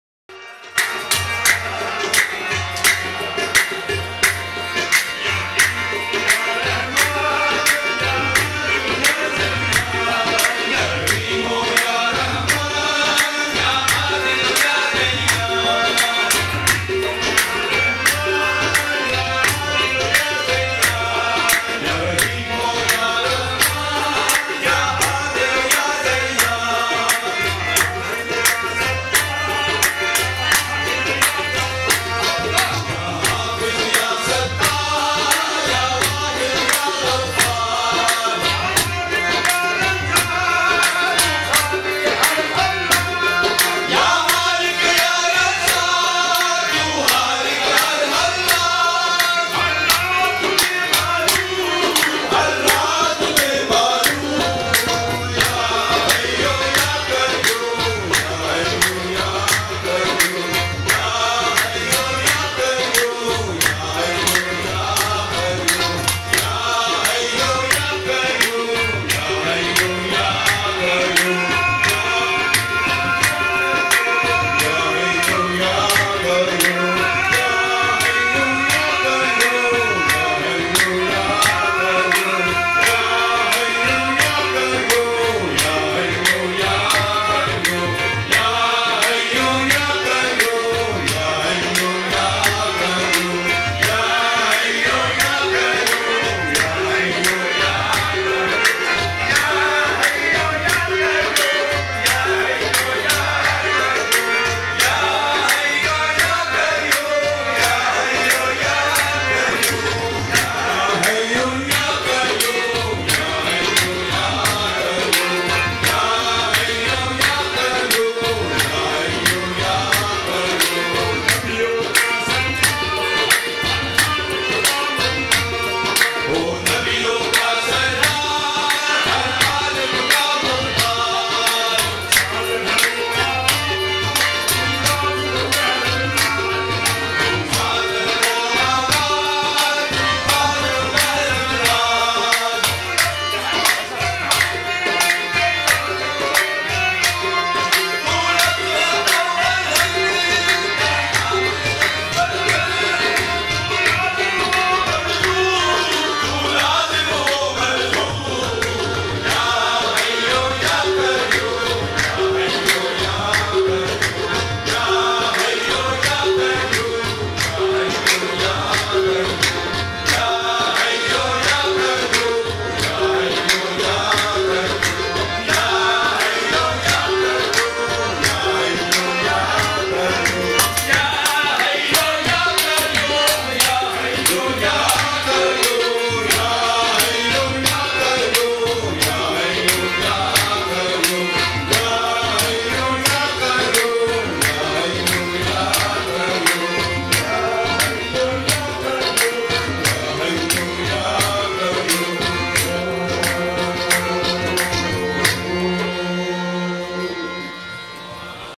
26/07/2017 Shahe Mardan Qawali with Sheikh Mehmet Adil
26/07/2017 Shahe Mardan Qawali group performing for honoured guest and teacher Sheikh Mehmet Adil in bradford.